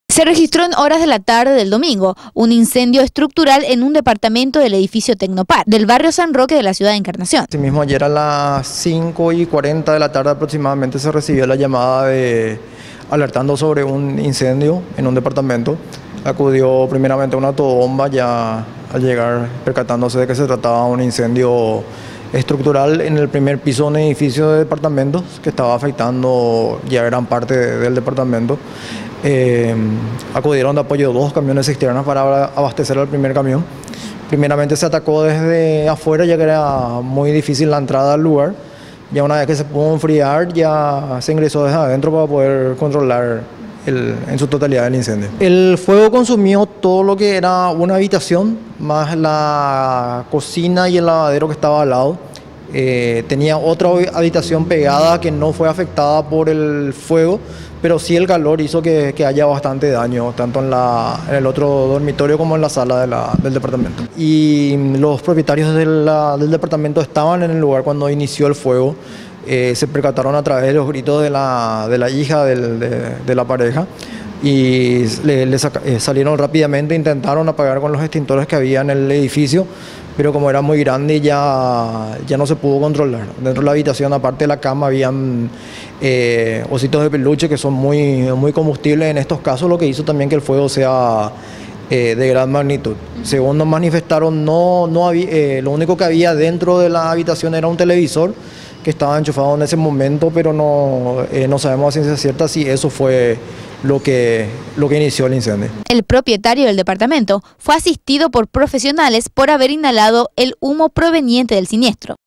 PRINCIPIO DE INCENDIO EN UN EDIFICIO EN B° SAN ROQUE
Principio de incendio en un departamento, del Edificio Tecnopar, del B° San Roque de esta ciudad ya controlado